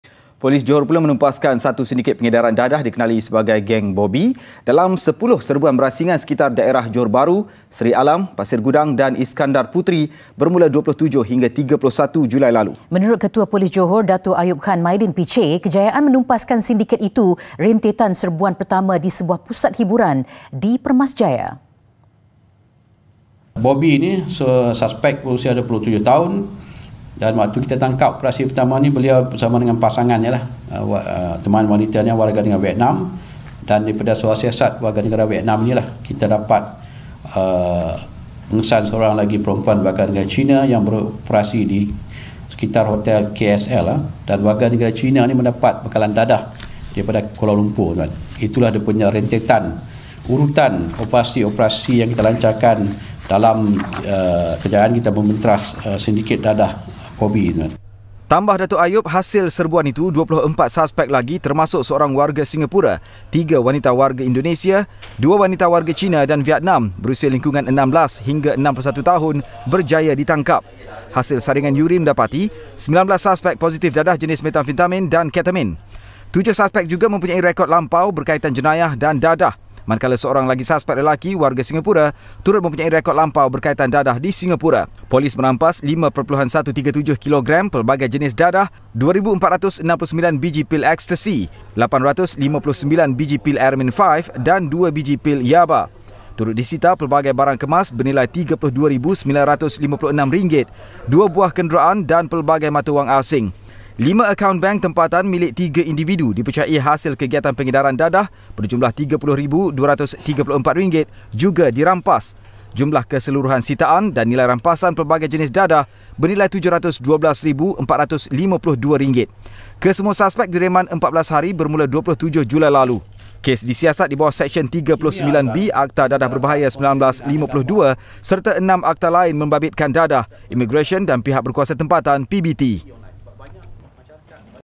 SINDIKET DADAH | Polis Johor Tumpaskan Geng Bobby
Ketua Polis Johor, Datuk Ayob Khan Mydin Pitchay berkata, kejayaan menumpaskan sindiket itu rentetan serbuan pertama di sebuah pusat hiburan di Permas Jaya.